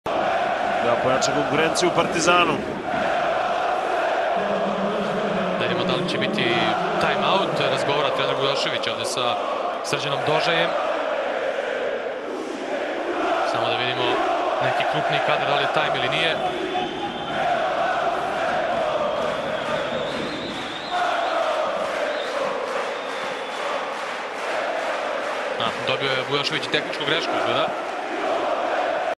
Dusko Vujosevic psuje
Dusko Vujosevic psuje na derbiju...Lako se moze raspoznati psovka :)